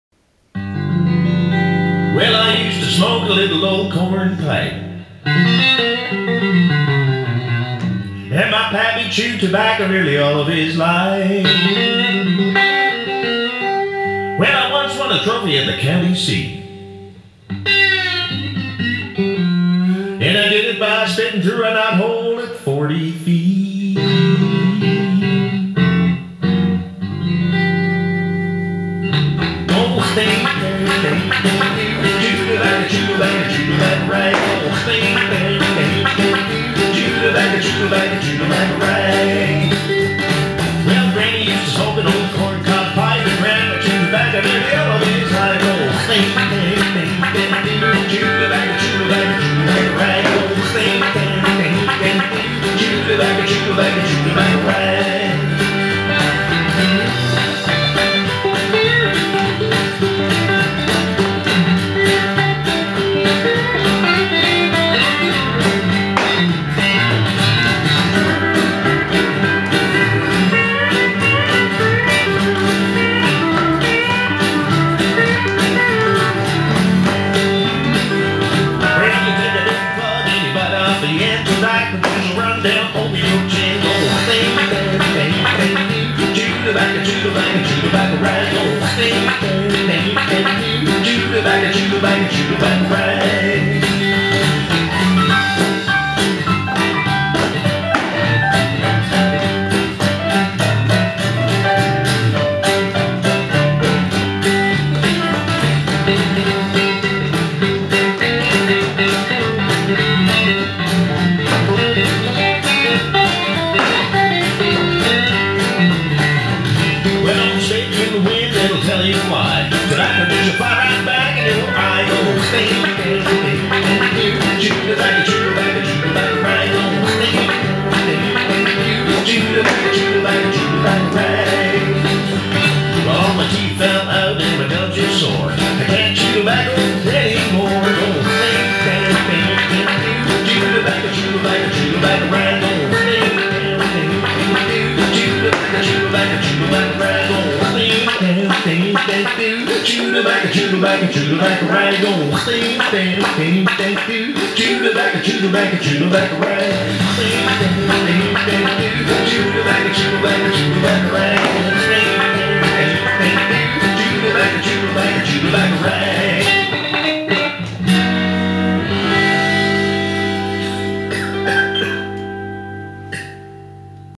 Chew Chew Spit
Linedance Country Style Catalan
Choreographer: Elisenda, Jesus, Santi, Veronica y Pepi
Description: Beginner, 56 count, 4 wall